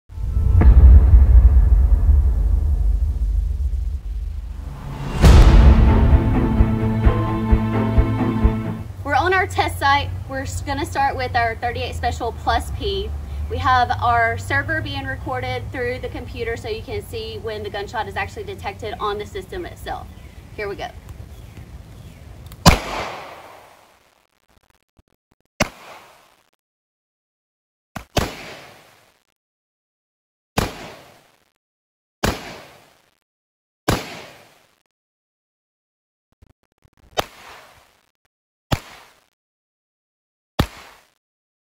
HALO Smart Sensor Demo wit HWPco
Mandi King tests HALO's gunshot detection in this demo.